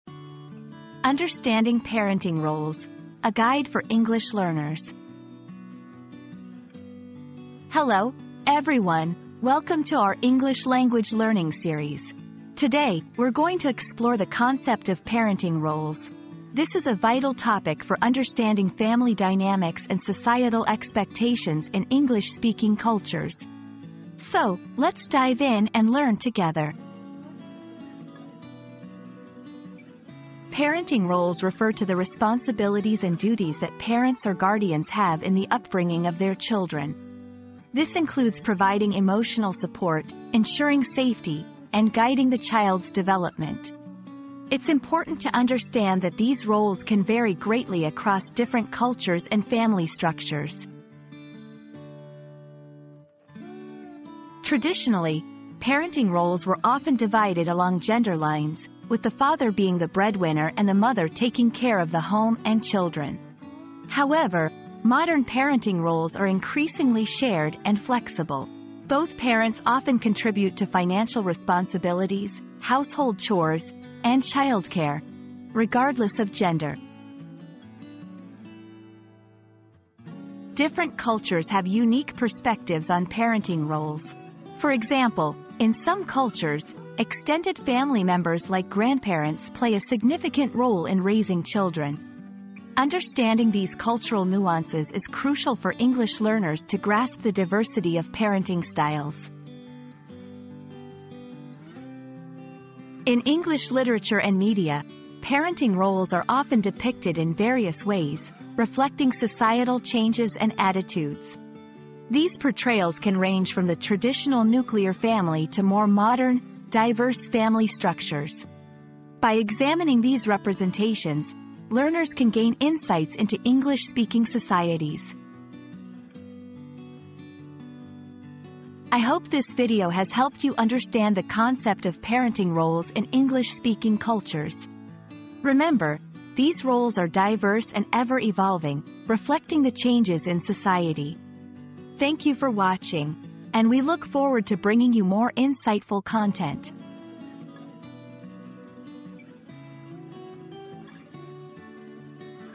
Understanding Parenting Roles: A Guide for English Learners
Unlocking Parenting Roles: A Guide for English Learners • Discover the essential guide for English learners to understand and navigate the complexities of parenting roles. Gain valuable insights and practical tips to enhance your parenting journey in a foreign language.

00:00 • Introduction - Understanding Parenting Roles: A Guide for English Learners
00:29 • Defining Parenting Roles
00:53 • Traditional vs. Modern Parenting Roles
01:19 • The Impact of Culture on Parenting
01:42 • Parenting Roles in Literature and Media